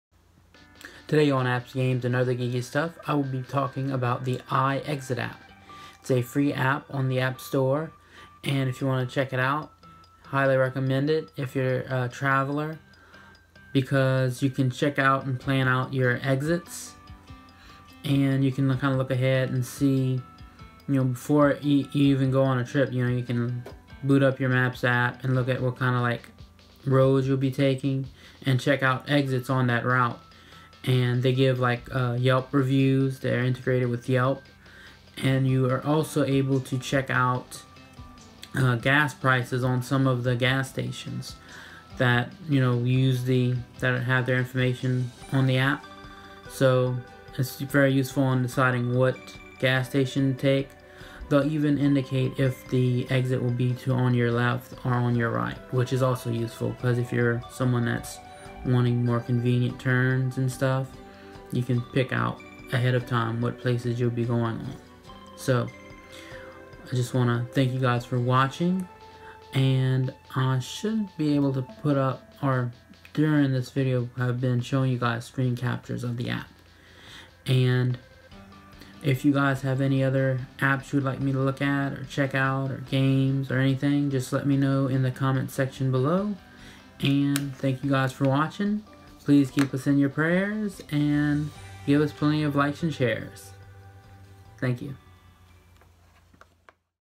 The iExit App
This apps, games, and other geeky stuff, I'll be talking about The iExit App an app that will help you pick some pitstops on road trips.